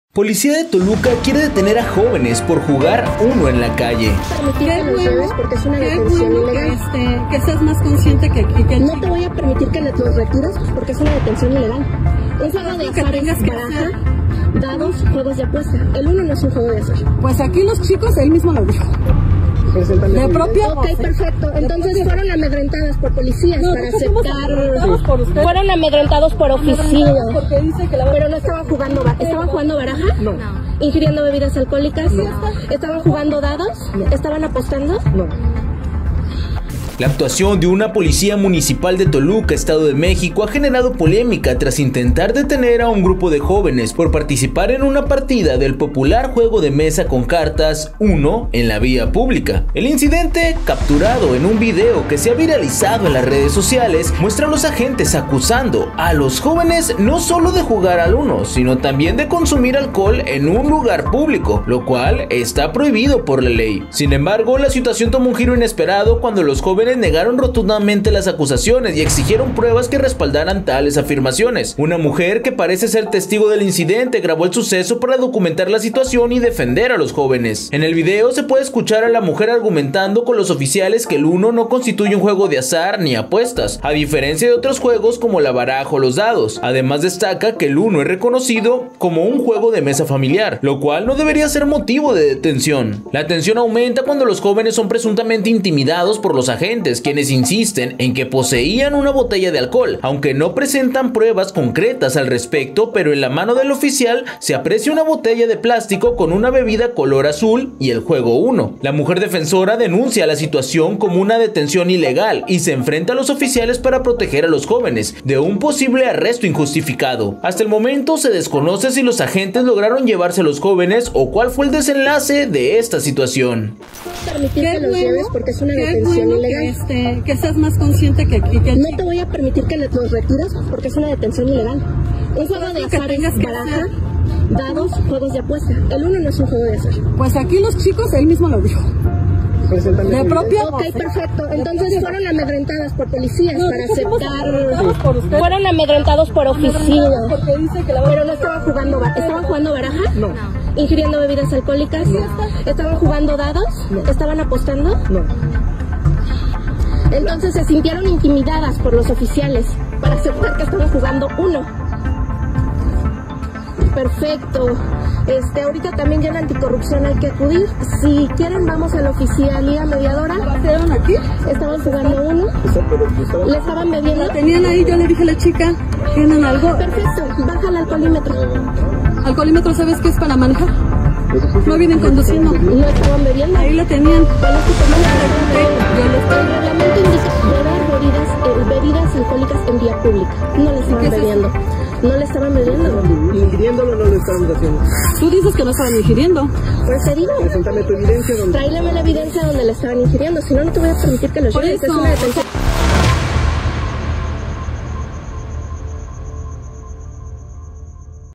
Policía de Toluca quiere detener a jóvenes por jugar UNO en la calle
La actuación de una policía municipal de Toluca, Estado de México, ha generado polémica tras intentar detener a un grupo de jóvenes por participar en una partida del popular juego de mesa con cartas "UNO" en la vía pública.